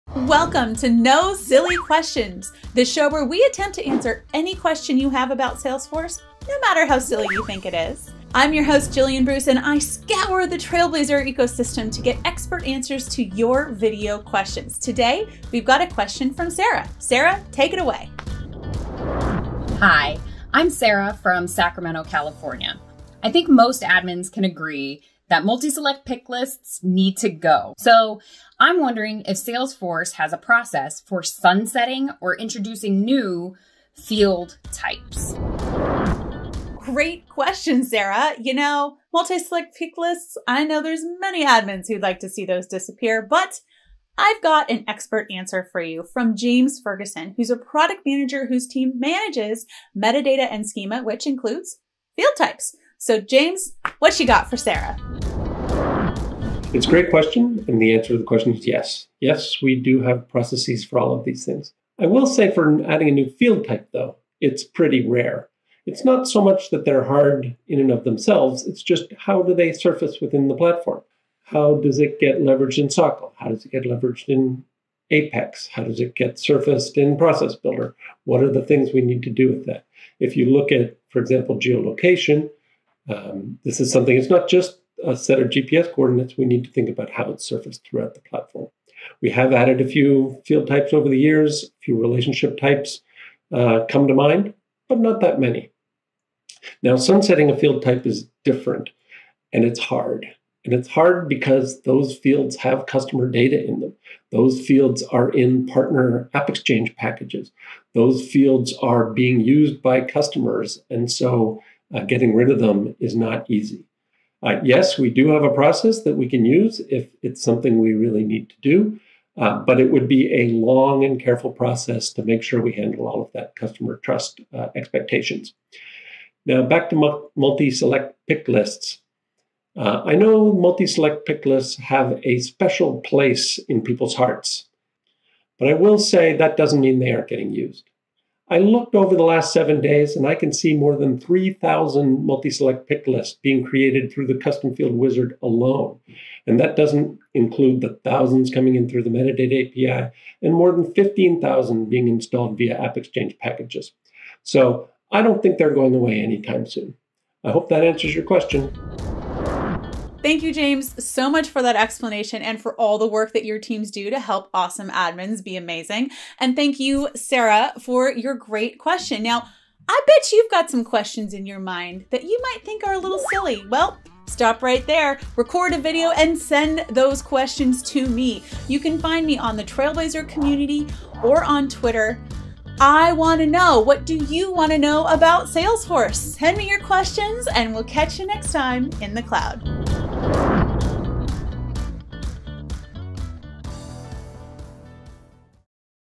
How and When Do Field Types Get Deprecated in Salesforce? | No Silly Questions
Trailblazer Community member Sarah Coulter asks, “How and when do field types get deprecated?” and gets an expert answer and demo from James Ferguson, Senior Director, Product Management and who’s team manages field types at Salesforce. Hosted by Gillian Bruce.